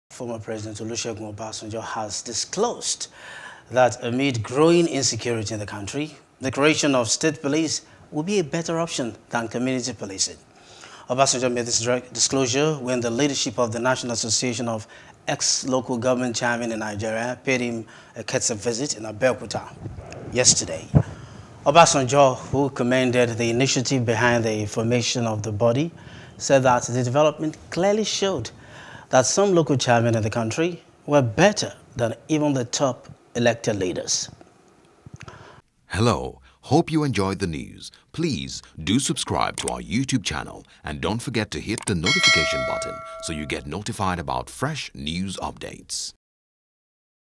Insecurity: State Police Now A Better Option - Obasanjo | NEWS
Former President Olusegun Obasanjo has disclosed that amid growing insecurity in the country, the creation of state police would be a better option than community policing.